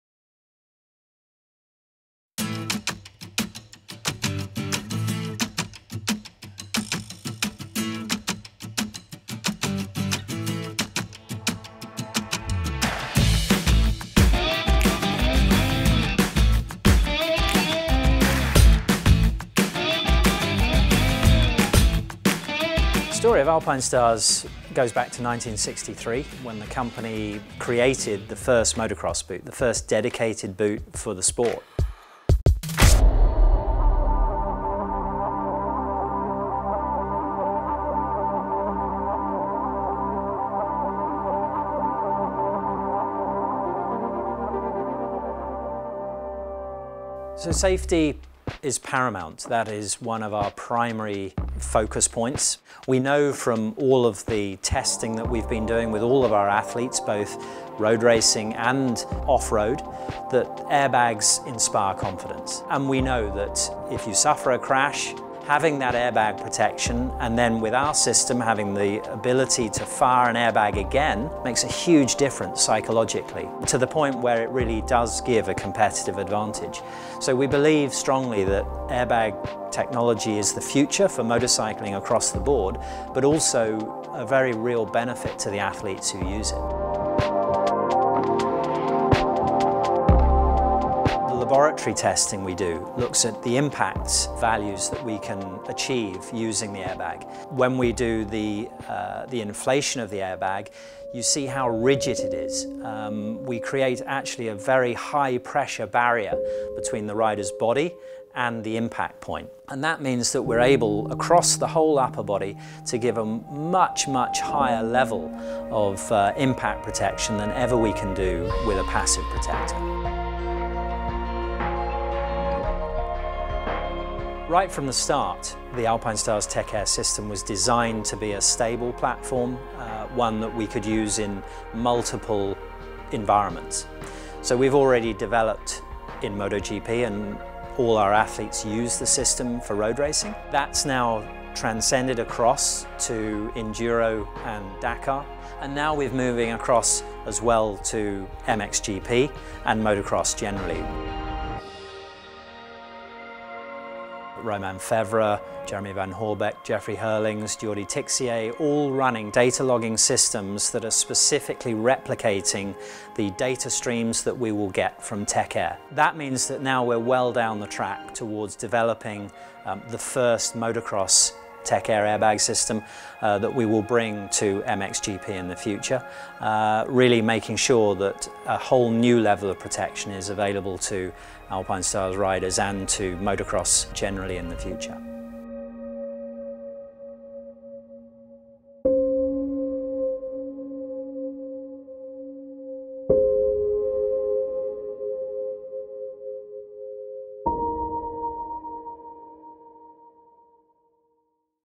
Tech-AIR for MX / Off-Road in the future
Many MXGP athletes are wearing data logging systems that are specifically replicating the data streams that we will be receiving from tech-Air. That means we are well down the track towards developing the first air airbag system for Motocross/Off-Road.